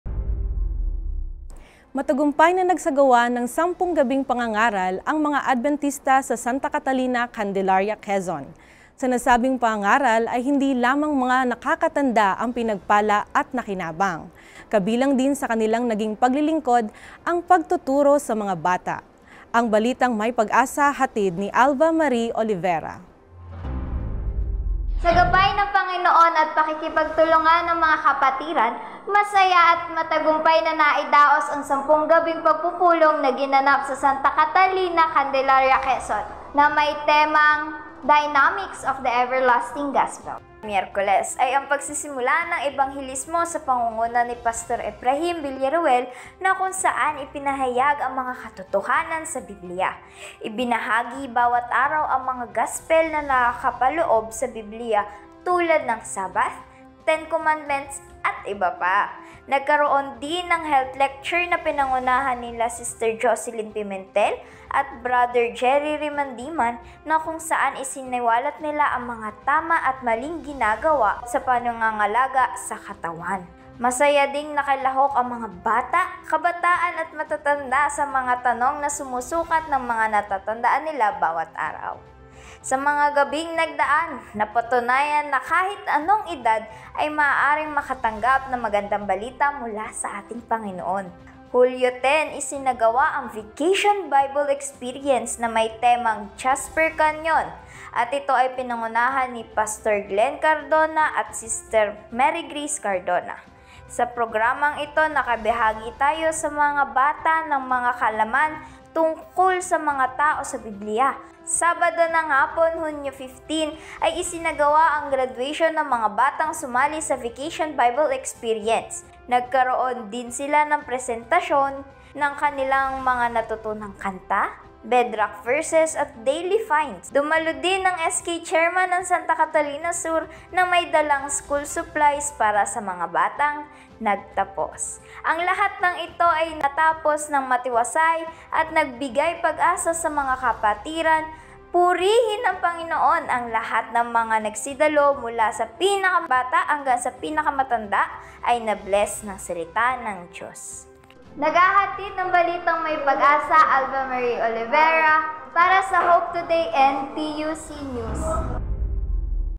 Bible and Health presentations, pagtuturo sa mga bata isinagawa ng mga Adbentista sa Sta. Catalina
Matagumpay na nagsagawa ng 10 gabing pangangaral ang mga Adbentista sa Sta. Catalina, Candelaria, Quezon.
Sa nasabing pangangaral ay hindi lamang mga nakakatanda ang pinagpala at nakinabang. Kabilang din sa kanilang naging paglilingkod ang pagtuturo sa mga bata.
Ang balitang may pag-asa hatid ni Alva Marie Olivera…